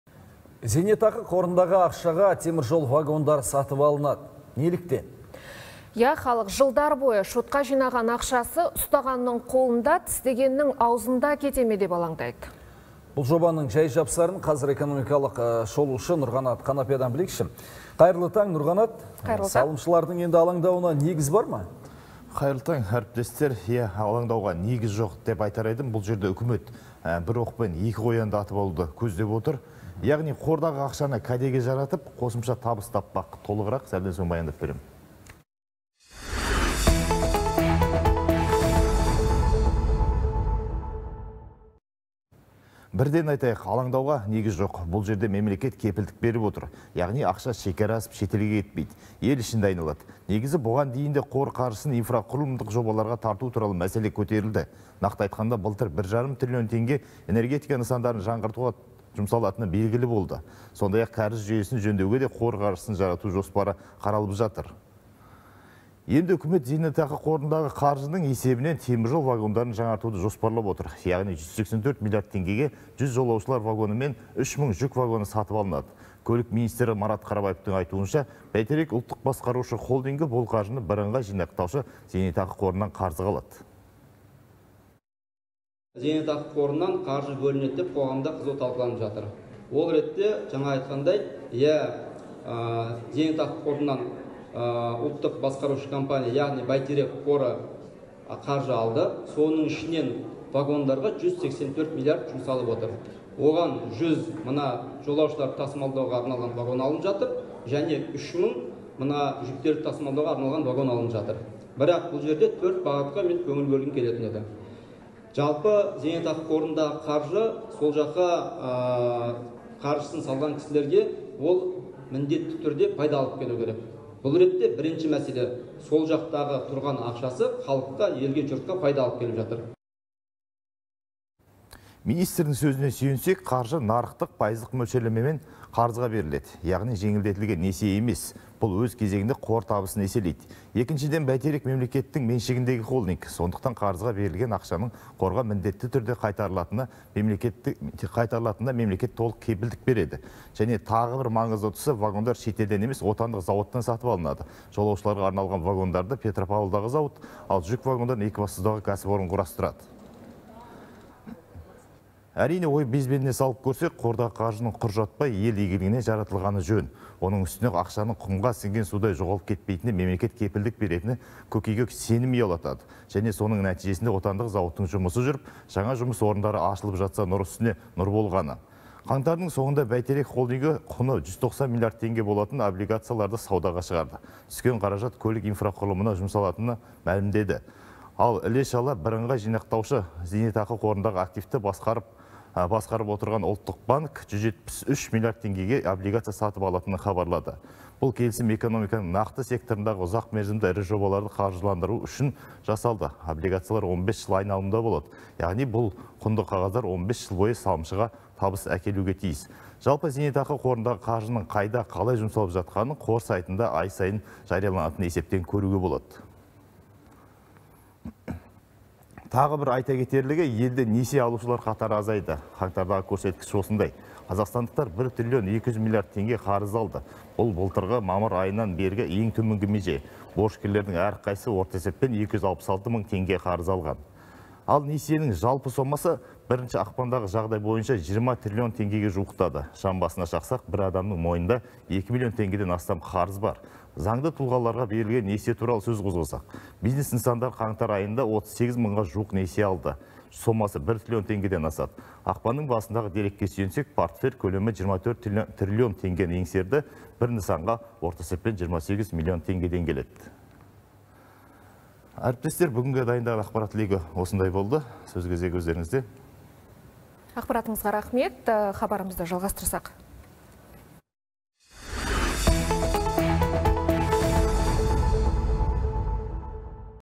Ақсақал, зейнетақыңызды қарызға алсақ деп едік?..
Зейнетақы қорындағы ақшаға теміржол вагондары сатып алынады. Неліктен?